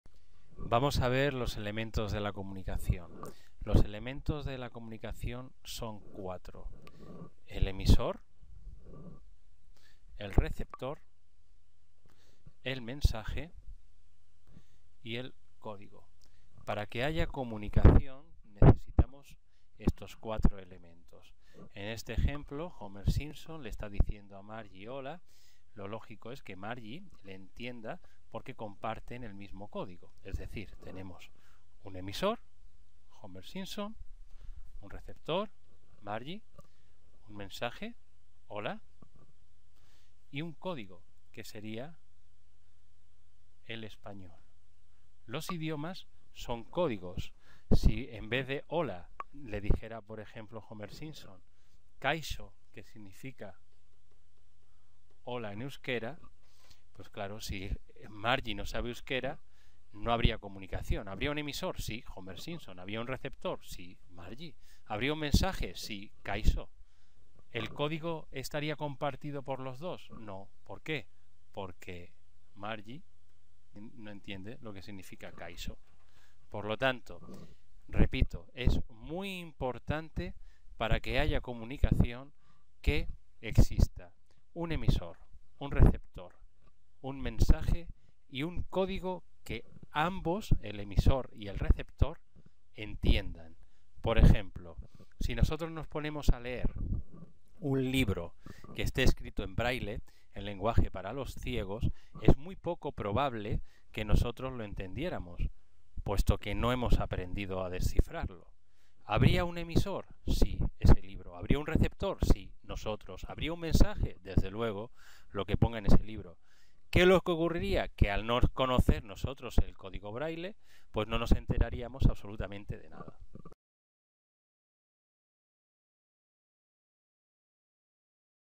La comunicación
Miguel Medel, profesor de Novaschool Añoreta, explica el proceso de comunicación para los alumnos de Primaria